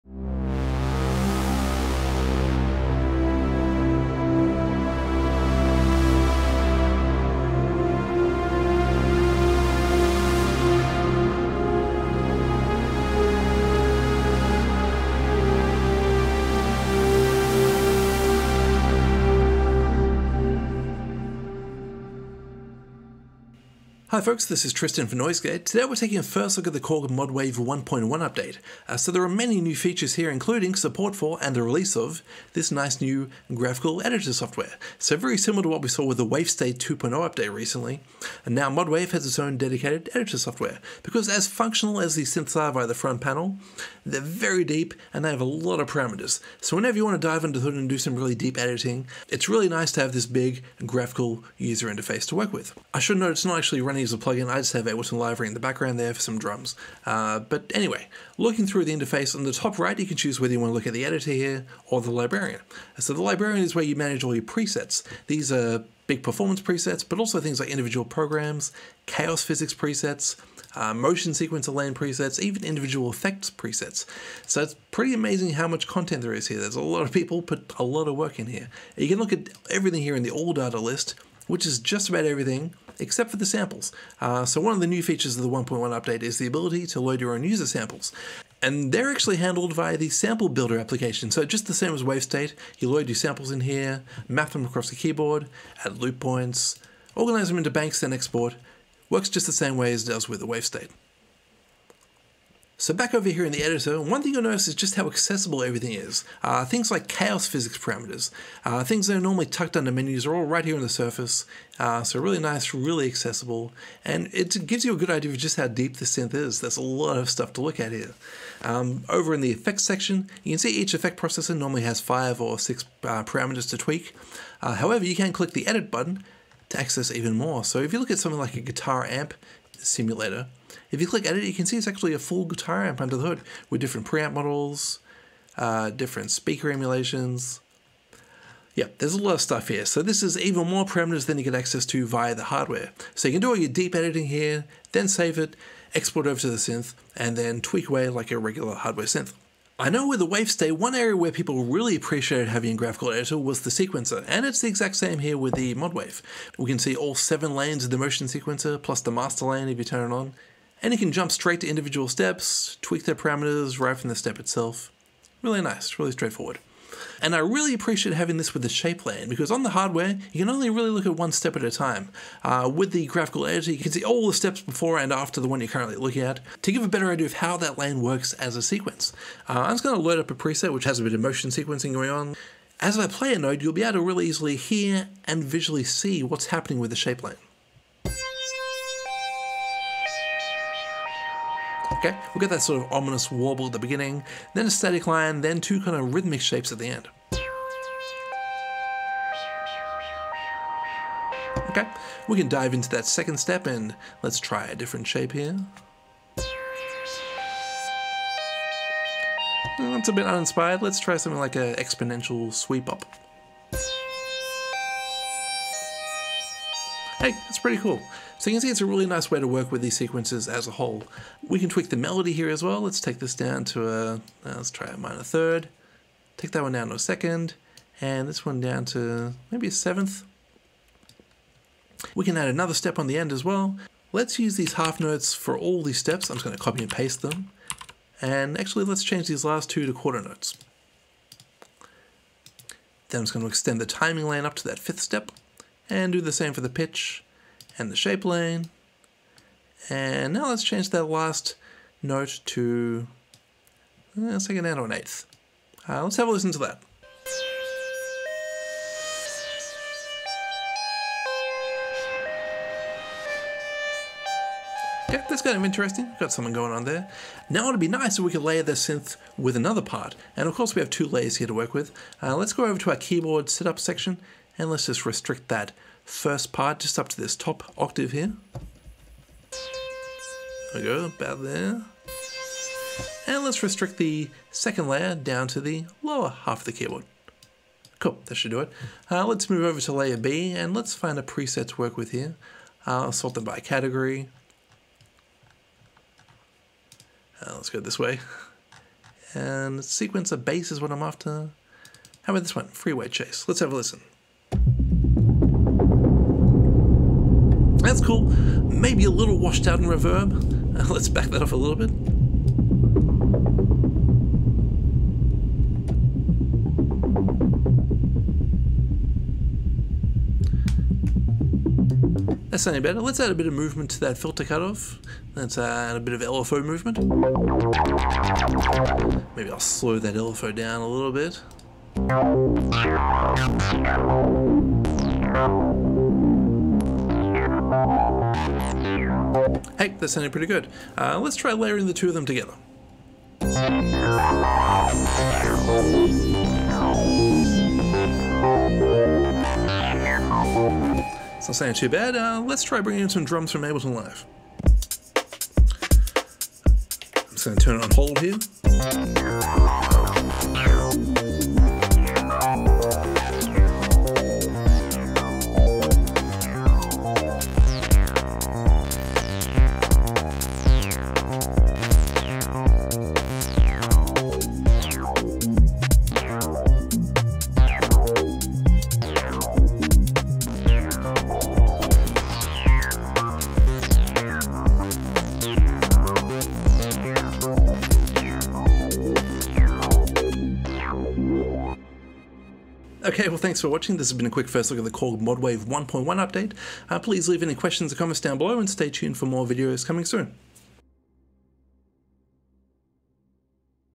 First Look - Korg modwave Editor Software and 1.1 Update
Tristan takes a look at the new editor/librarian software for Korg's modwave synth,